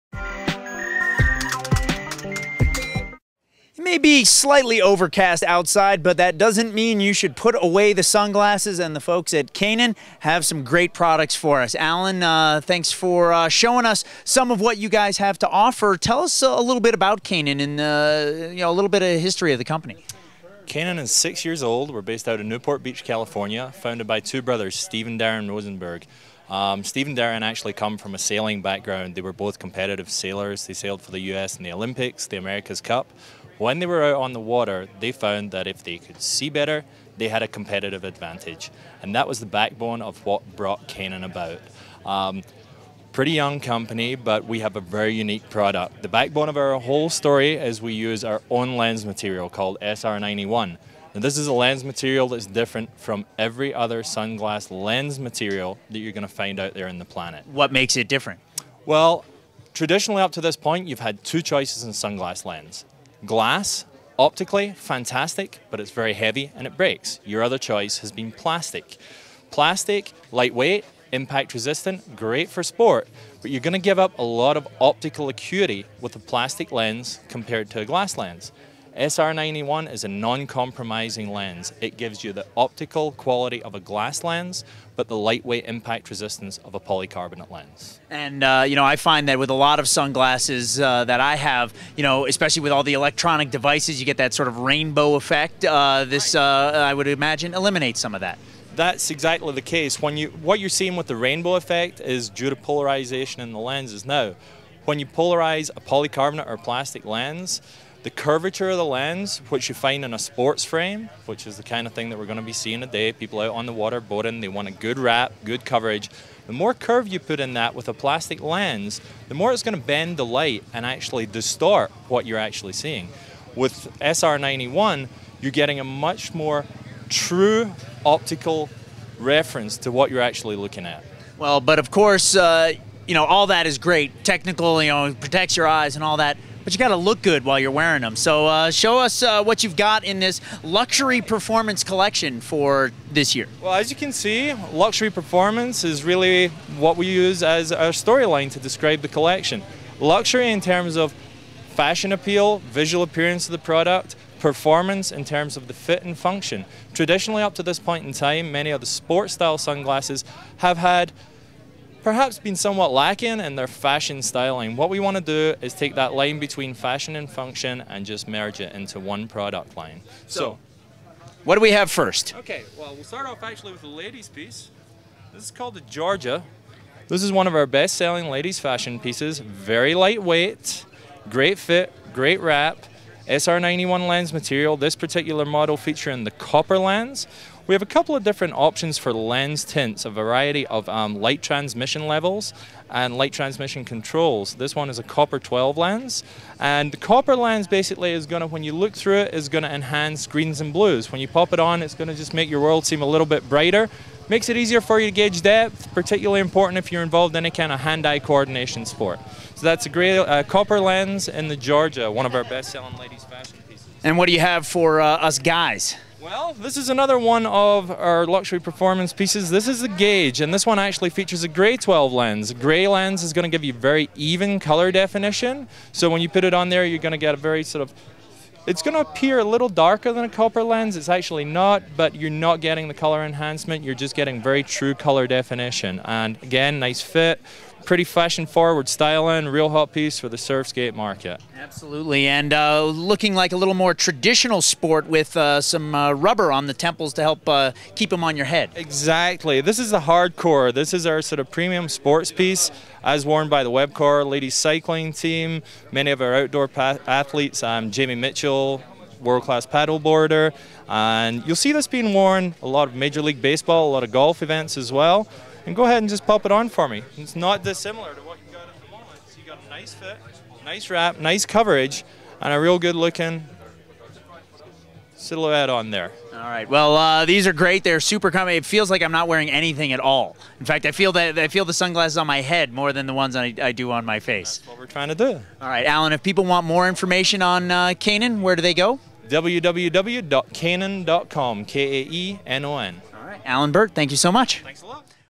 Outdoor Retailer '08: Kaenon Sunglasses
Developed by former America's Cup sailors, Kaenon makes shades that are fashionable and functional.